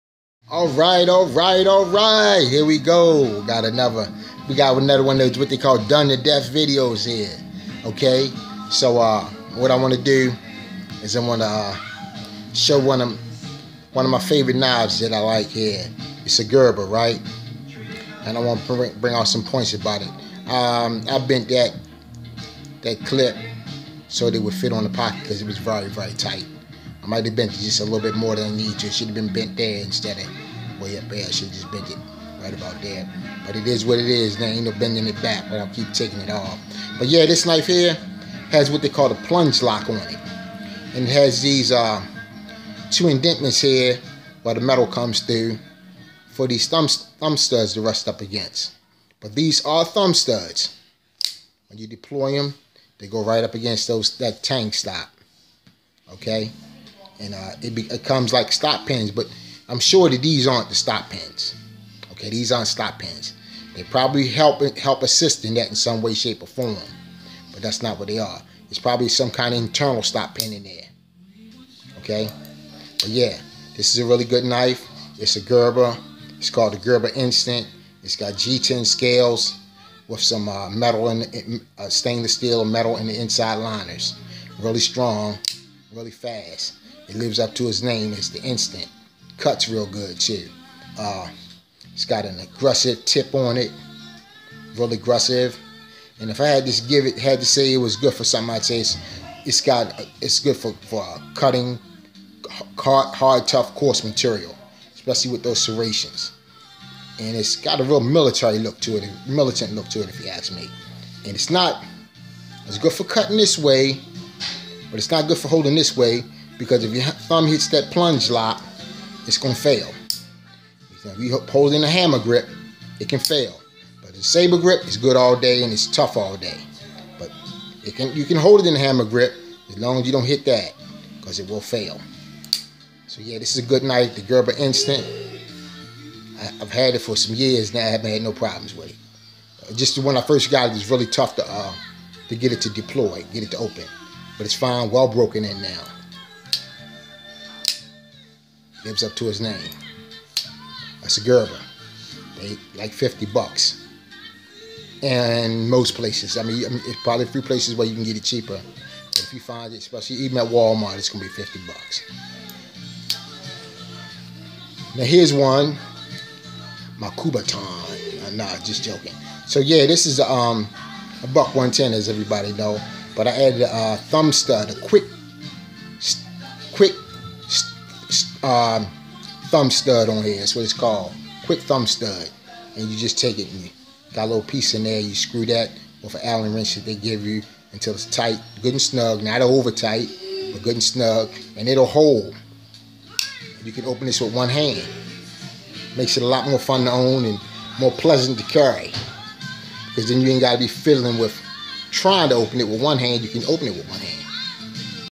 Gerber Instant a fast deploying folder Part 1 of 3
This is the GERBER INSTANT pocket knife a quality folder that has held up as I hoped it would and it still has Gerber warranty to back it up.